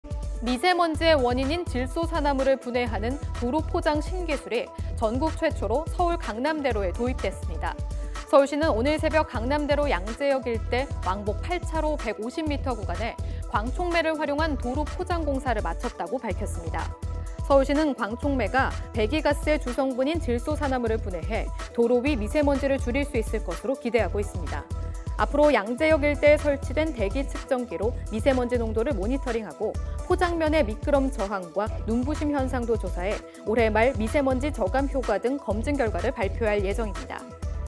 '광촉매' 도로 포장...미세먼지 저감 기대 / YTN 사이언스
미세먼지의 원인인 질소산화물을 분해하는 도로포장 신기술이 전국 최초로 서울 강남대로에 도입됐습니다.

서울시는 오늘 새벽 강남대로 양재역 일대 왕복 8차로 150m 구간에 '광촉매'를 활용한 도로 포장 공사를 마쳤다고 밝혔습니다.

서울시는 광촉매가 배기가스의 주성분인 질소산화물을 분해해, 도로 위 미세먼지를 줄일 수 있을 것으로 기대하고 있습니다.

앞으로 양재역 일대에 설치된 대기측정기로 미세먼지 농도를 모니터링하고, 포장면의 미끄럼 저항과 눈부심 현상도 조사해 올해 말 미세먼지 저감효과 등 검증 결과를 발표할 예정입니다.